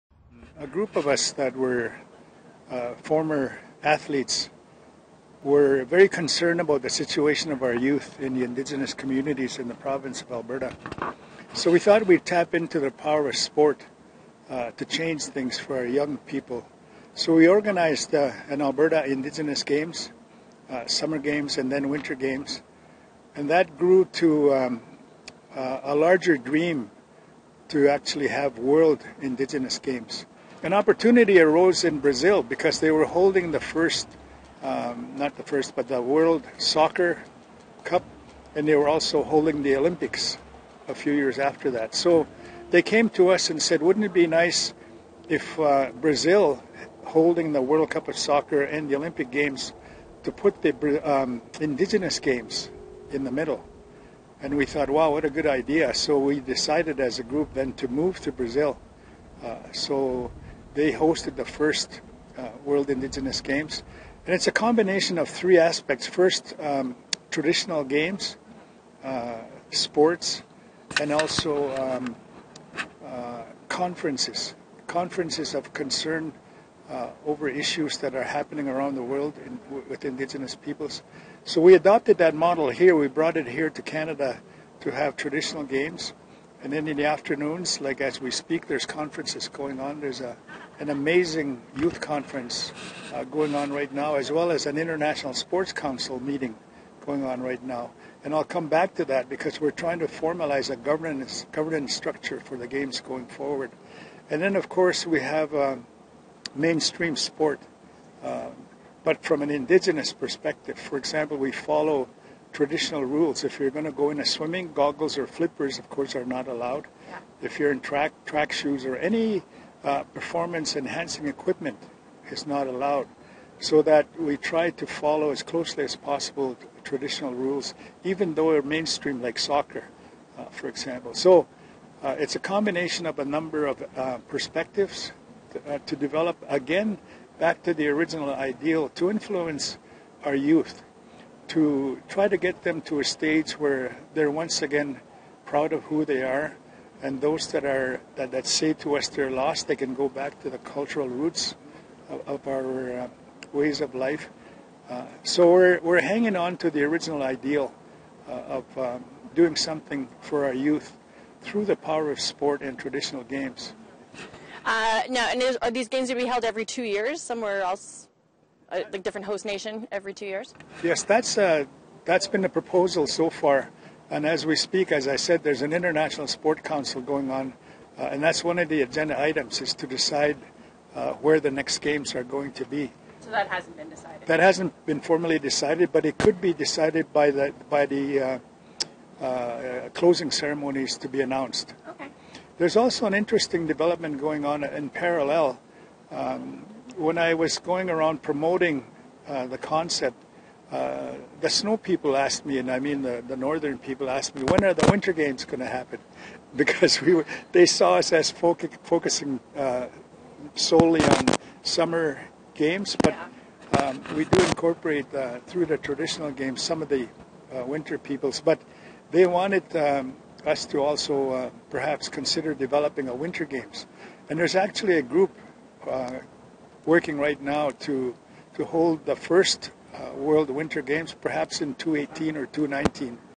Grand Chief Willie Littlechild Explain His Vision for the WIN Games | WIN Games 2017
Grand Chief Willie Littlechild is credited with bringing the WIN Games here.

He sat down with APTN to explain his vision and the work that went into making it all happen.